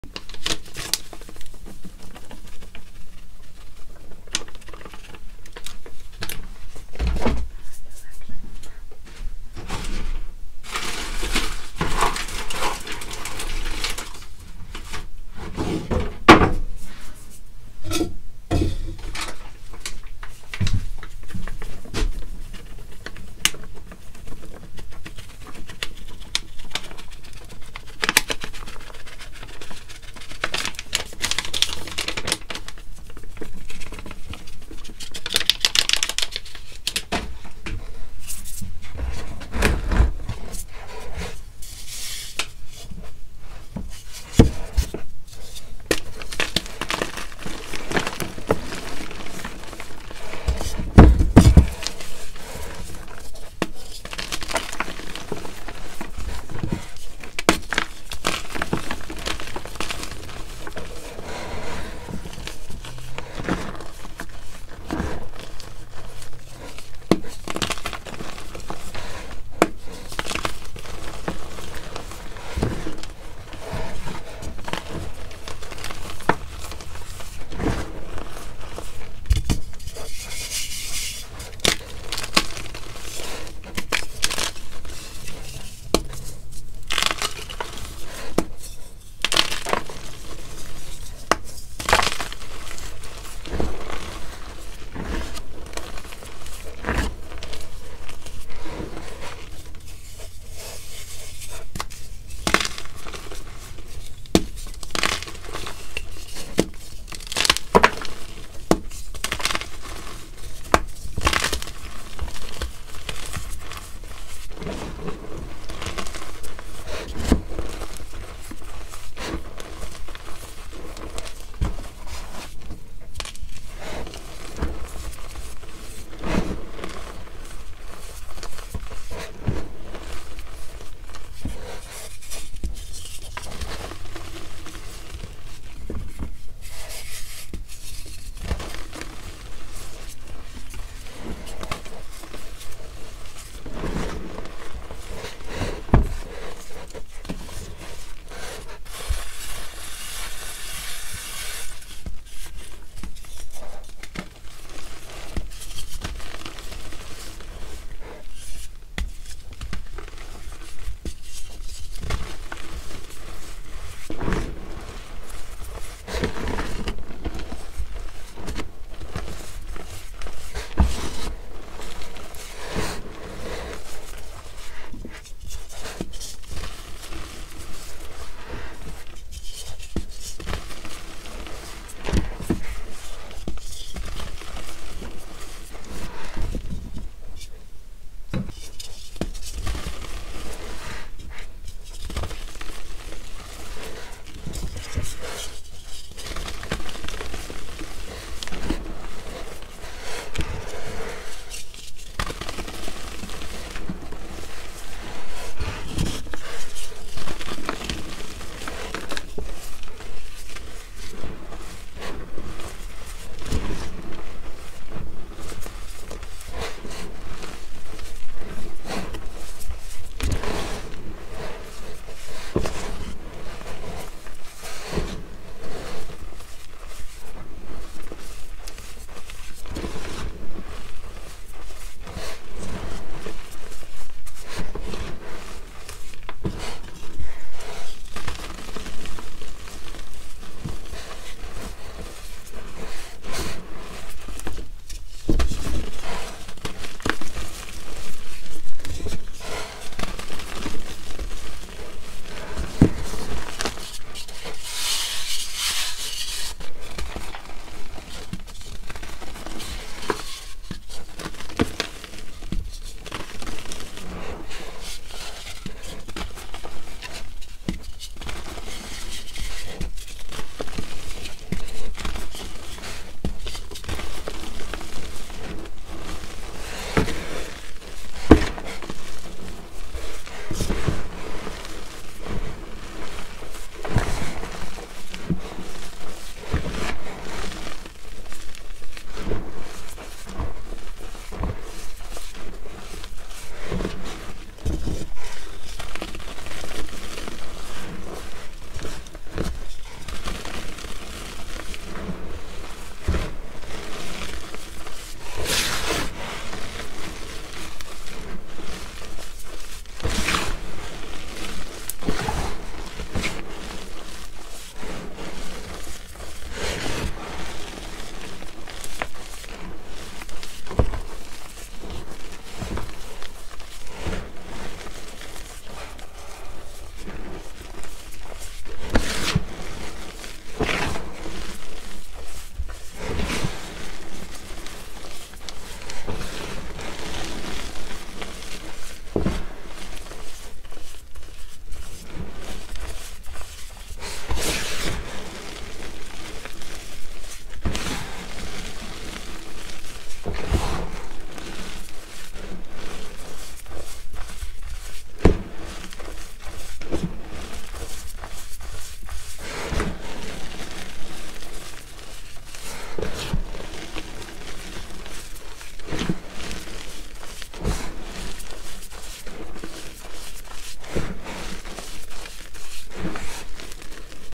Love for the Moon
I’m horrible with titles lol. So got red hearts, a red moon and 4 blocks of Chalkness with red on top.

Absolutely no posting/editing of my content on any platform!

Thank you for all of your love and support! I appreciate you all so so much.